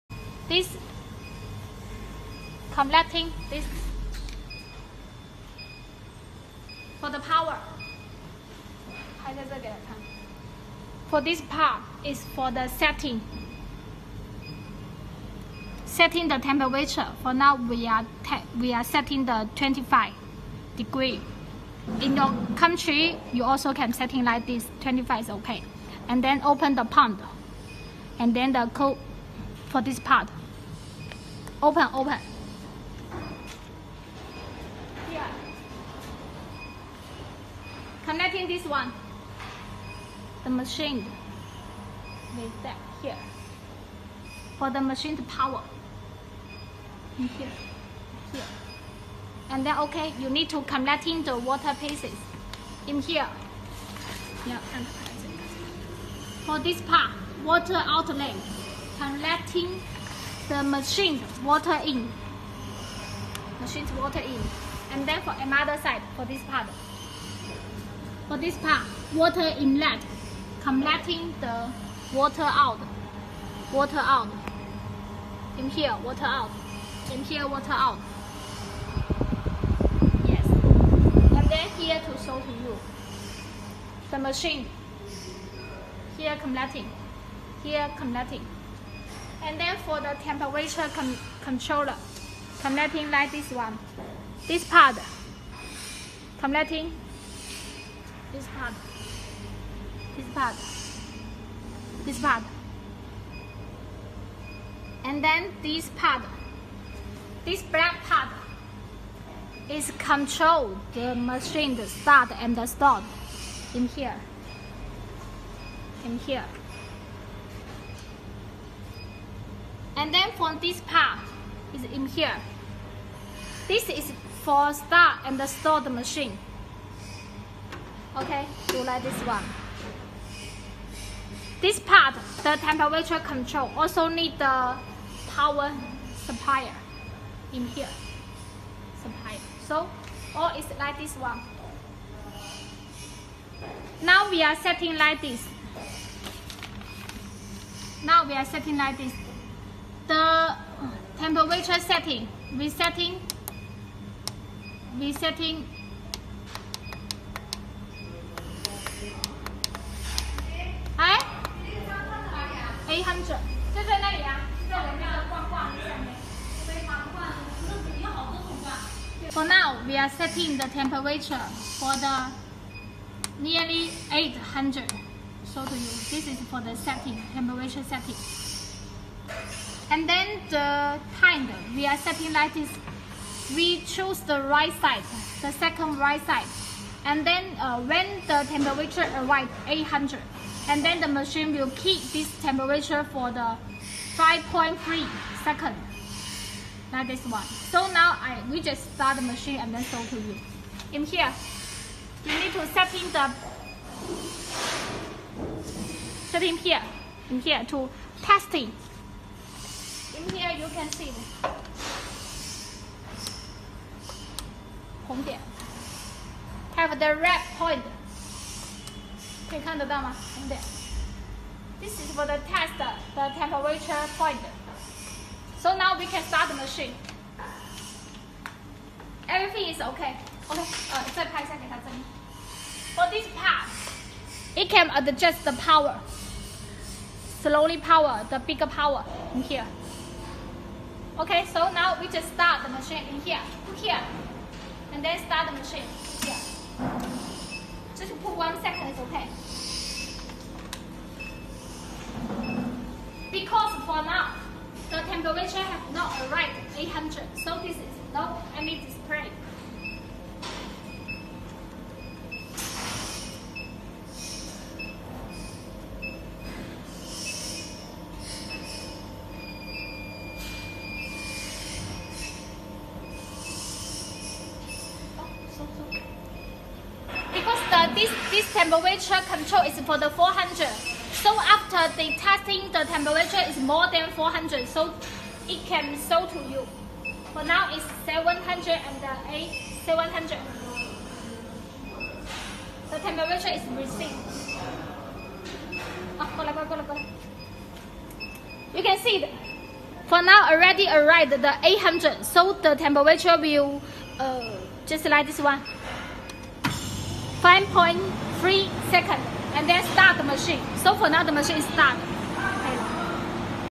How to Connect Induction Heating Machine And Water Chiller And Temperature Controller
JL-40KW with JL-AC-6HP